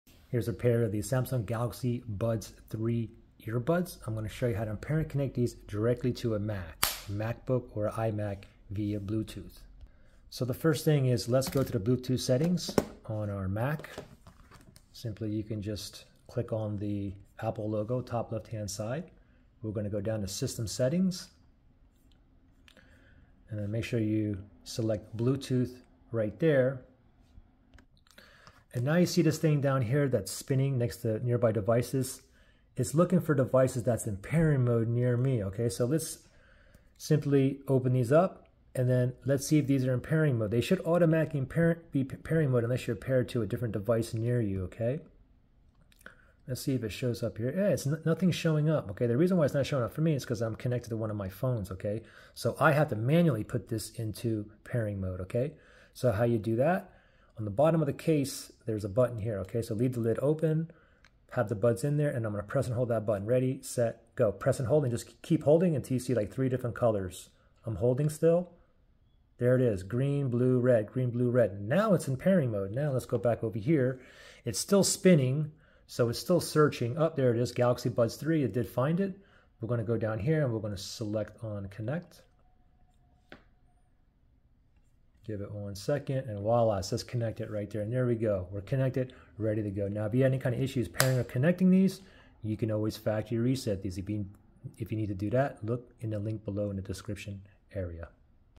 Galaxy Buds 3 Earbuds: How to Connect to a Mac (MacBook / iMac)
I show you how to pair and connect the Samsung Galaxy Buds 3 earbuds to an Apple Mac (MacBook / iMac) via Bluetooth wireless connection. I hope this helps.

Should you have paring or connecting issues then try a factory reset on the Galaxy Buds 3

Samsung Galaxy Buds 3 Wireless Earbud Headphones